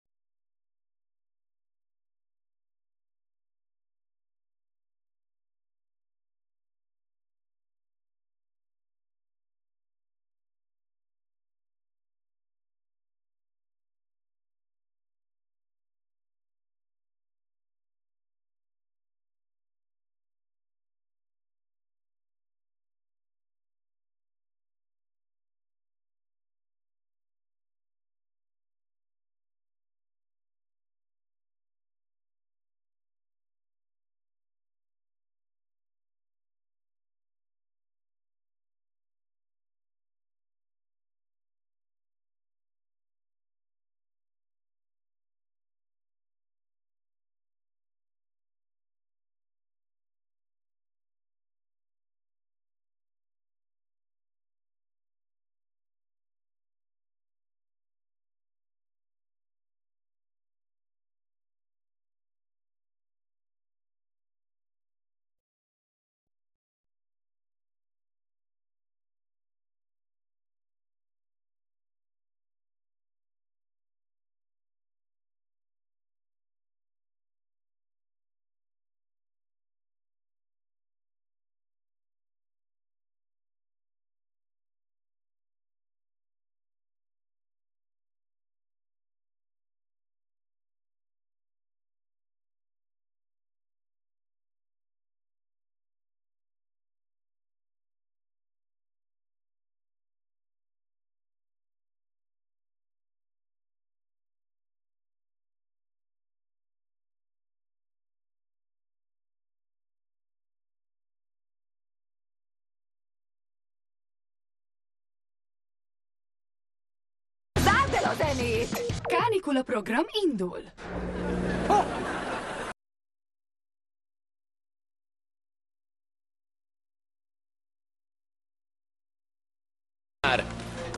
TeenNick Hungary Technical Difficulties (2022. 07. 06)
Just imagine a blank image with any animation or audio just suddenly appears, and it stares at your dead soul with lacking context when you were watching one of your favorite Nickelodeon shows in...Hungary, for some reason...It was at like 7 or 8 pm CET as well so extra spoopy
btw this video is not available in canada because of because

Uploaded for uploaded for